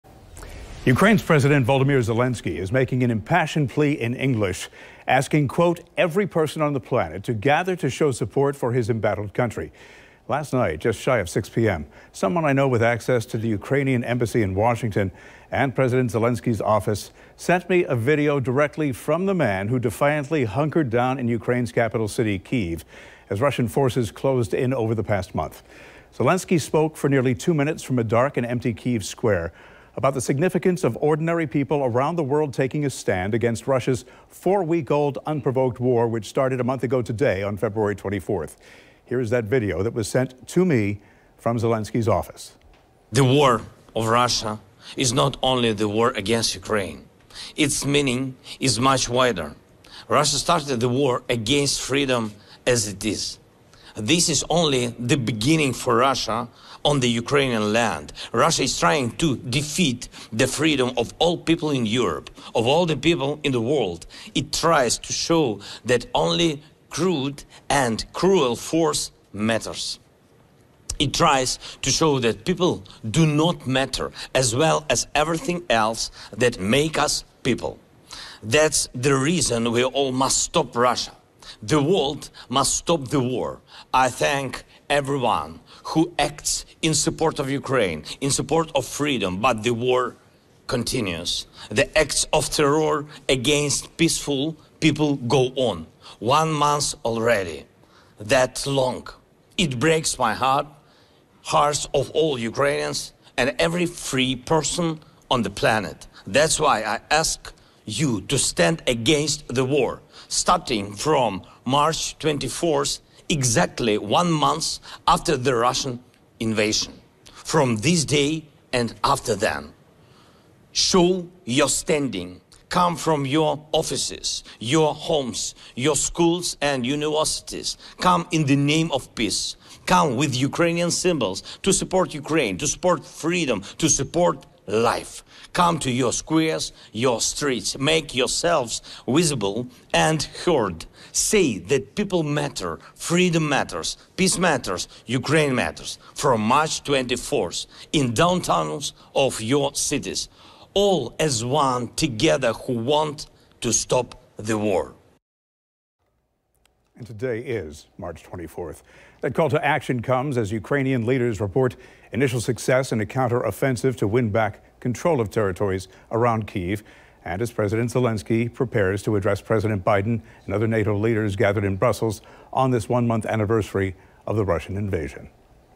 Ukraine's President Voldomyr Zelenskyy global address
Ukraine's President Voldomyr Zelenskyy is making an impassioned plea in English asking quote "every person on the planet" to gather to show support for his embattled country.